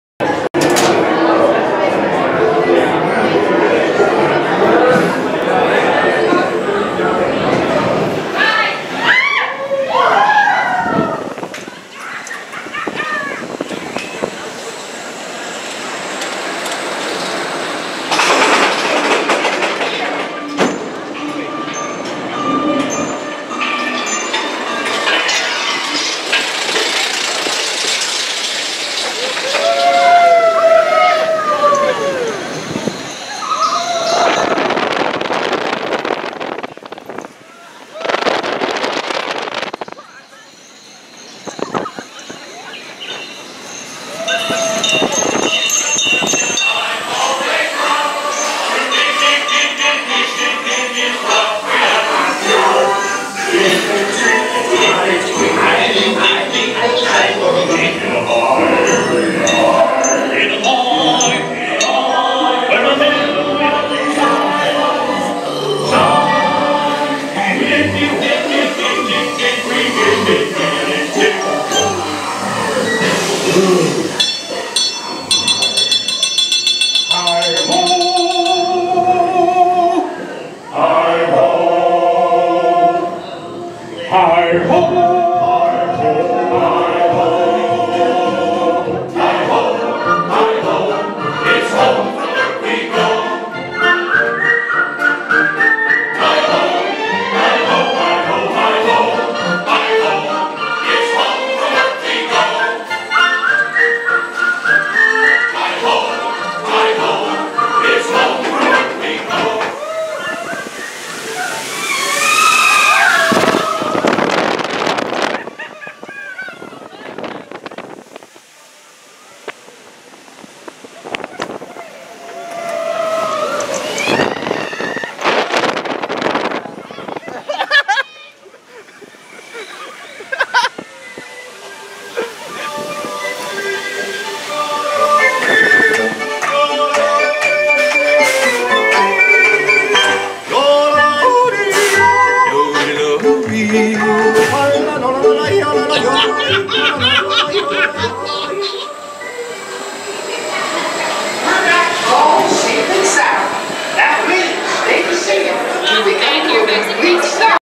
Seven Dwarfs Mine Train Ride Through
One of our ride-through videos of the Seven Dwarfs Mine Train